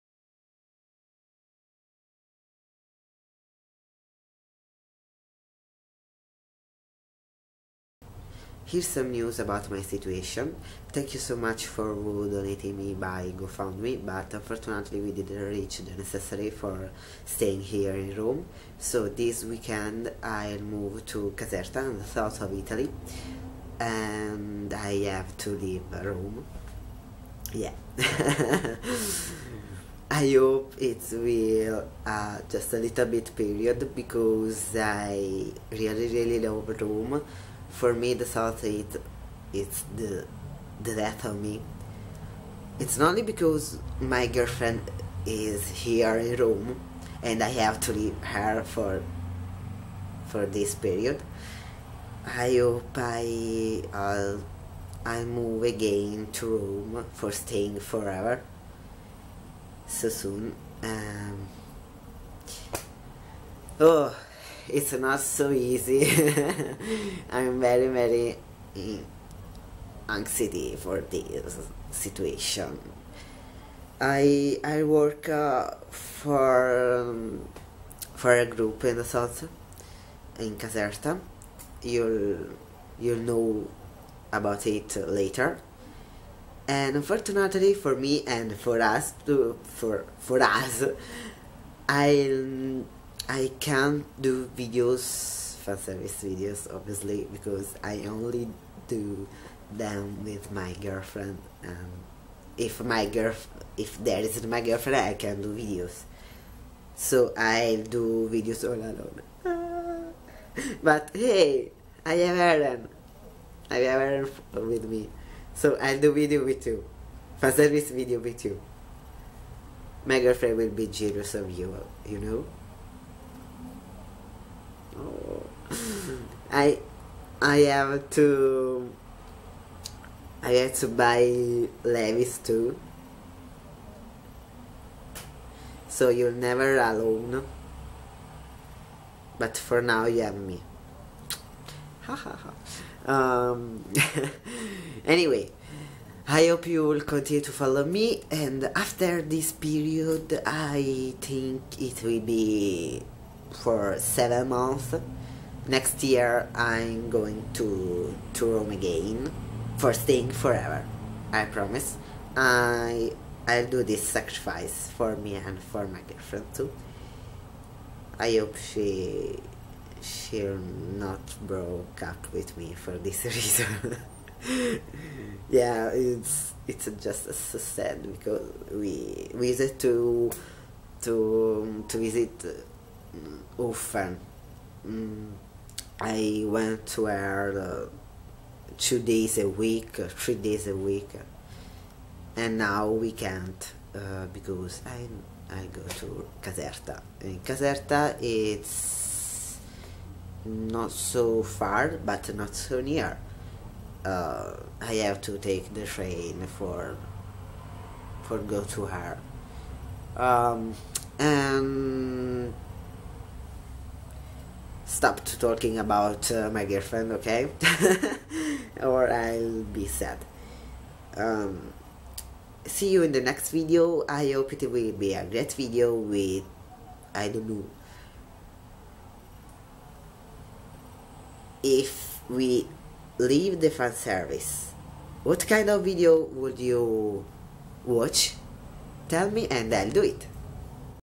Video log: Bye bye Rome
Don't be sad for this news. give me your support! I will need it! Every nice comments will be appreciated in a moment like this ç_ç
I hope I will give you soon good news! My projects for my future will never stop! I'll continue with cosplay and with my life's goals, for me and for my dearly beloved

Continue to support! Thank you soooo much!